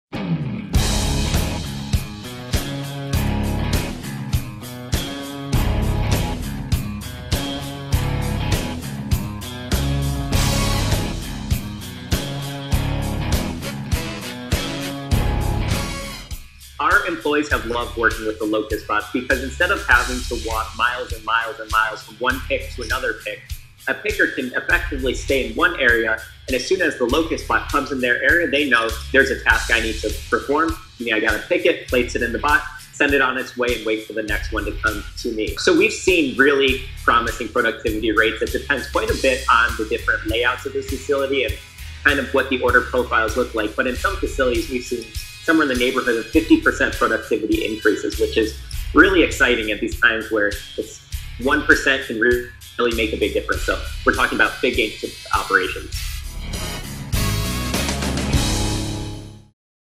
See DHL Robots at Work: LocusBots
LocusBots travel through many of the DHL warehouses, saving warehouse associates from walking extra miles each day. The LocusBots travel to them, pick up items for orders and move on. The results? Higher productivity and happier associates.